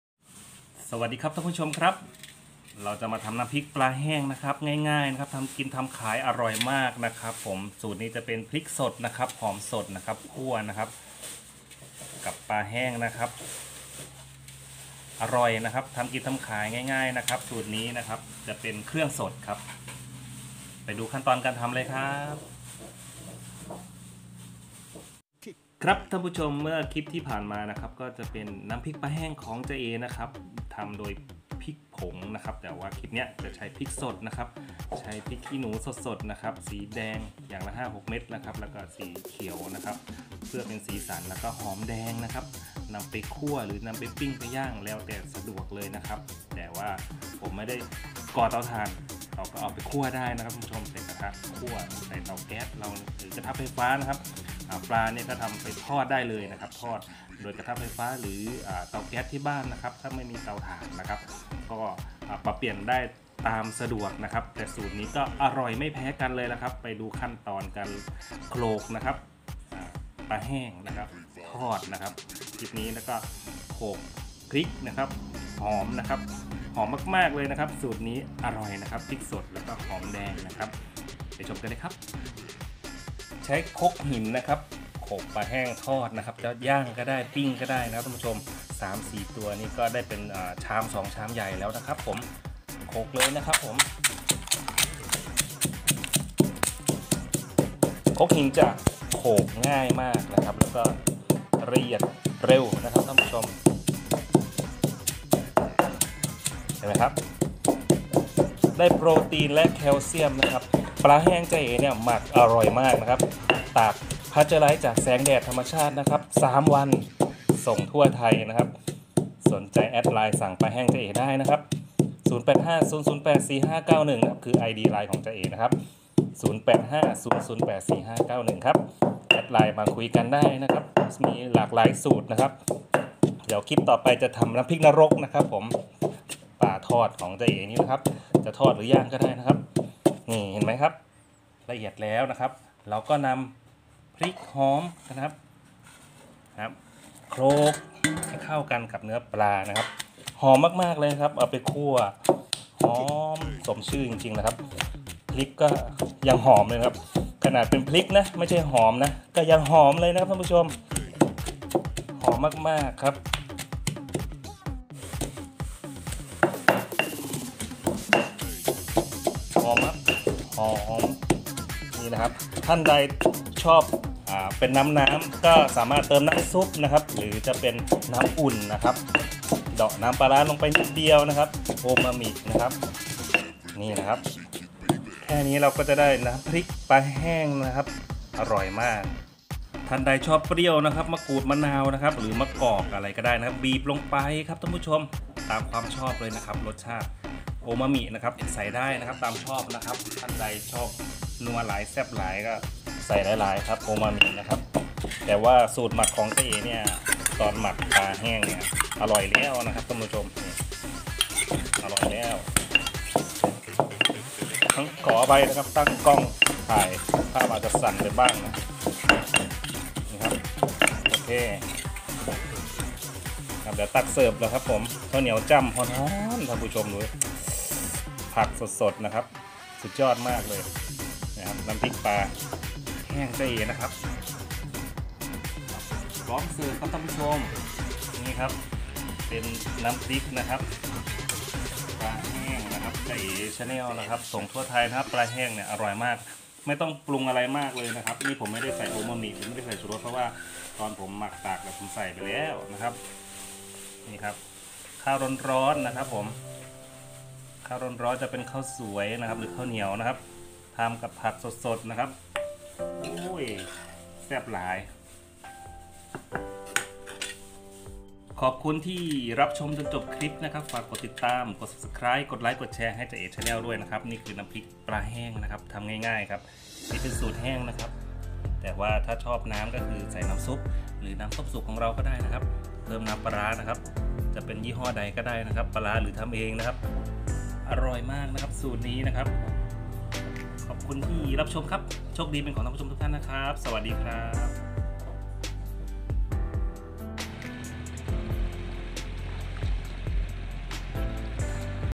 วิธีทำน้ำพริกปลาแห้ง ทำกิน ทำขาย ง่ายๆ อร่อยมากๆ
วิธีทำน้ำพริกปลาแห้ง ทำกิน ทำขาย ทำง่าย อร่อยมากๆ